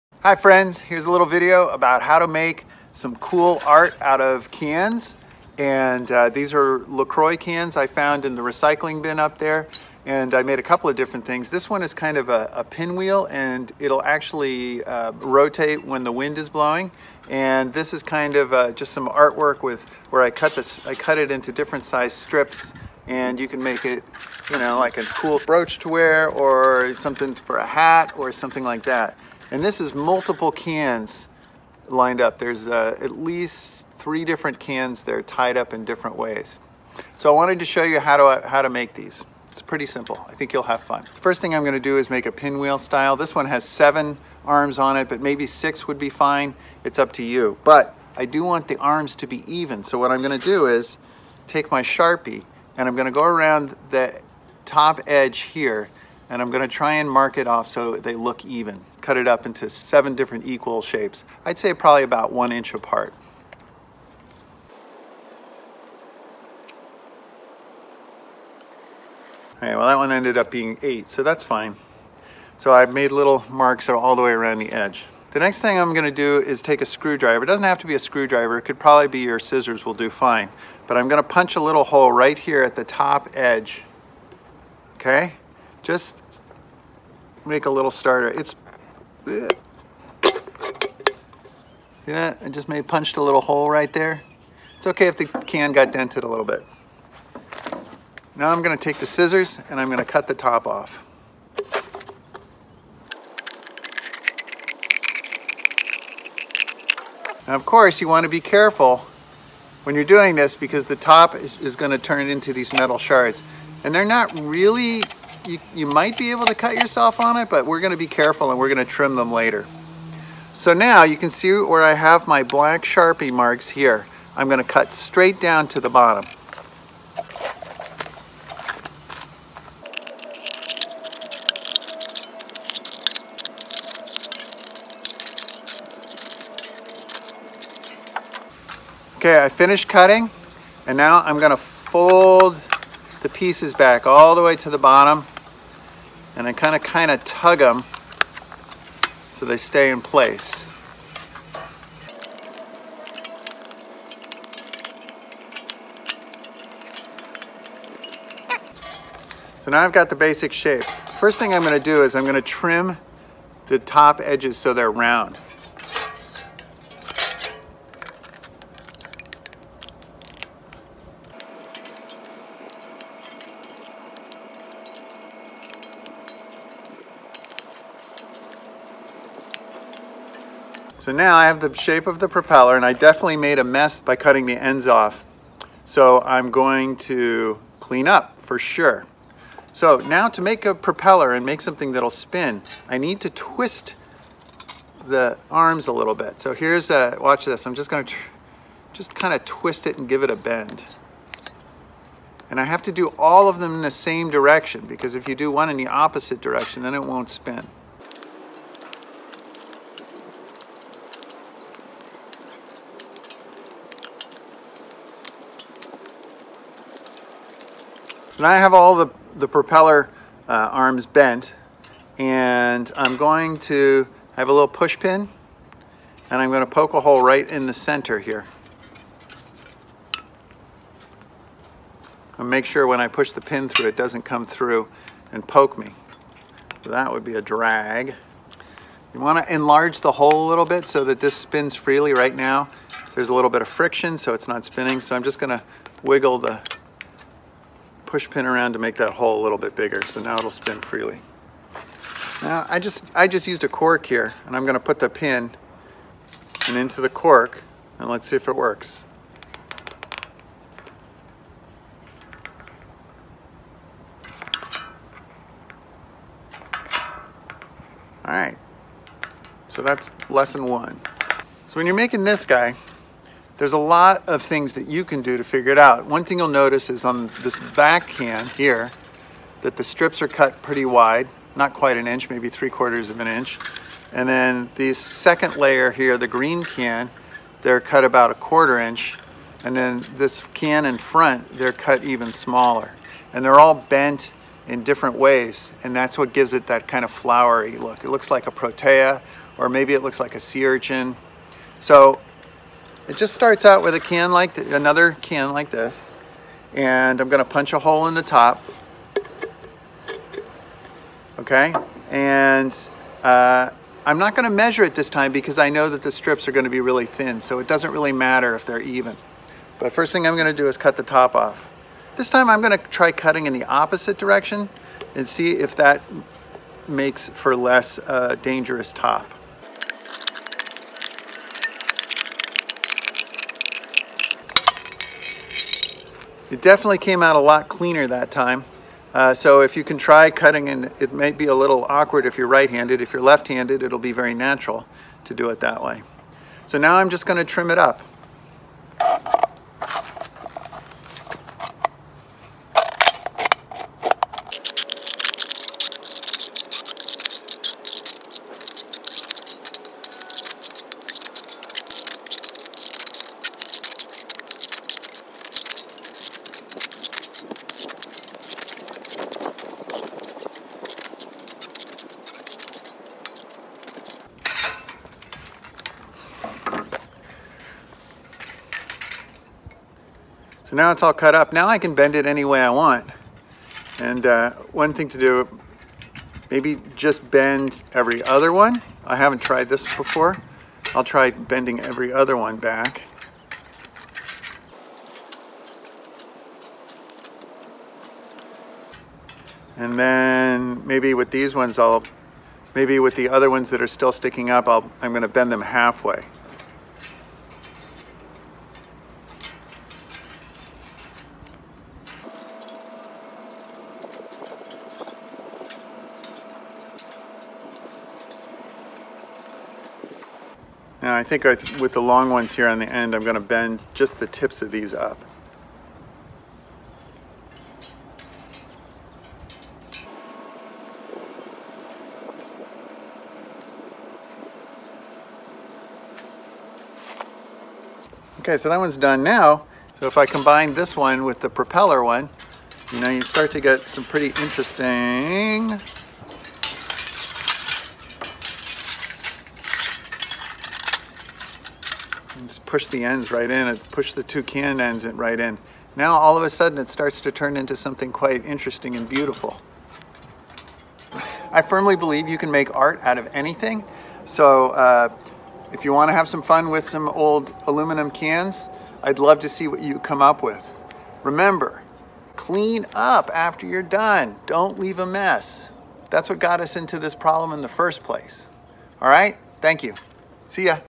Trashion- Aluminum Can Art
How to make art by cutting up aluminum cans.